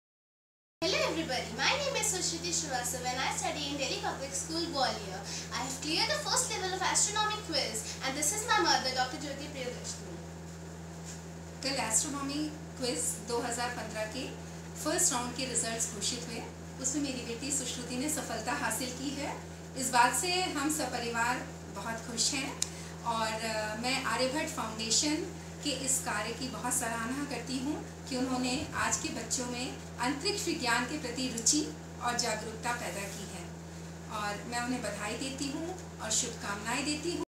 Sushruti Shrivastava selected in Prelims 2015-16
Sushruti Shrivastava from DPS Gwalior shares her experience on getting selected in Preliminary level of Astro Quiz.